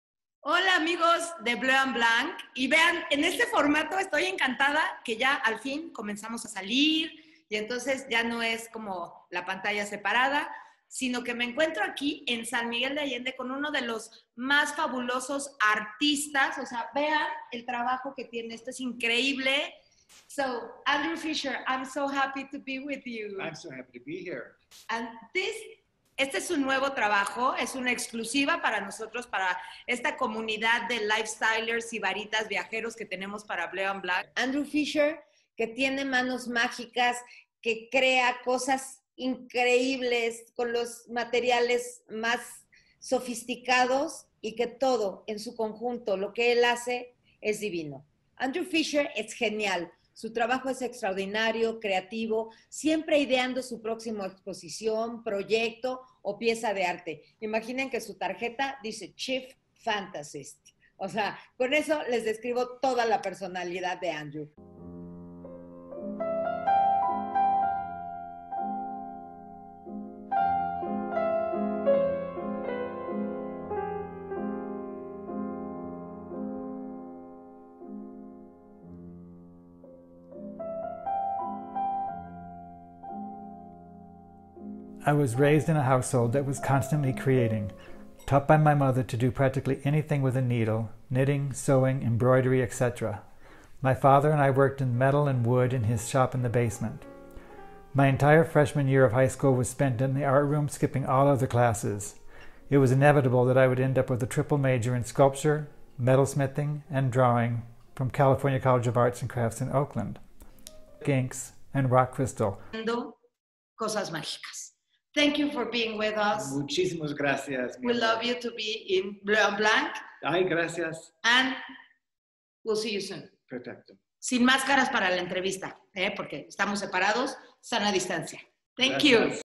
Rose Ruiz entrevista a Andrew Fisher, Chief Fantastic en Fisher Weiseman Collection para Bleu&Blanc.
Es un honor que Andrew Fisher me recibiera en su Imagine Gallery en San Miguel de Allende, Chief Fantastic me recibió en una deliciosa mañana en donde tomamos café y hablamos sobre su arte, esta ciudad Corazón de México, sus galerías, restaurantes y más.

Andrew es genial, su trabajo es extraordinario, creativo, siempre ideando su próxima exposición, proyecto o pieza de arte, su tarjeta de presentación dice Chief Fantasist. Soy muy inquieto me dice, como una ardilla, mi mamá siempre me tenía ocupadas las manos, es por eso que hago tantas manualidades, ¡y lo hace perfecto¡

En su estudio, Imagine Gallery en San Miguel de Allende trabaja siete días a la semana y nunca se detiene para almorzar. Aprovechando su experiencia como escultor y joyero, diseña piezas originales para los clientes de la firma y también es el cerebro del desarrollo creativo de la Colección Fisher Weisman de muebles, accesorios e iluminación

En su práctica artística de estudio, Fisher es igualmente prolífico, creando complejos tapices  de hoja de oro cosidos a mano que traen la calidez del sol a las paredes de cualquier museo, galería o casa.

El sello distintivo de Fisher Weisman, como se describe en la revista dominical del New York Times, es “drop dead, elegant chic".

Andrew Fisher y Jeffry Weisman combinan imaginación desenfrenada y una sensibilidad artística única con un enfoque de diseño muy sobrio y atención al detalle. Los interiores y el mobiliario que conciben son individuales, duraderos y memorables.

Andrew Fisher obtuvo una licenciatura en artes del metal, dibujo y escultura en el California College of the Arts y luego diseñó interiores con John Hallock, creó instalaciones de fantasía con Tony Duquette y se estableció como un artista fino con seguidores leales. (vea los tapices y esculturas dorados de Andrew en andrewfisherart.com).

Jeffry Weisman obtuvo una licenciatura en Diseño y un MBA en la Universidad de Stanford. Después de diseñar interiores corporativos en Skidmore, Owings & Merrill y Gensler, estableció su propia práctica para desarrollar y licenciar diseños de productos con Charles Pfister, y luego se expandió a interiores residenciales.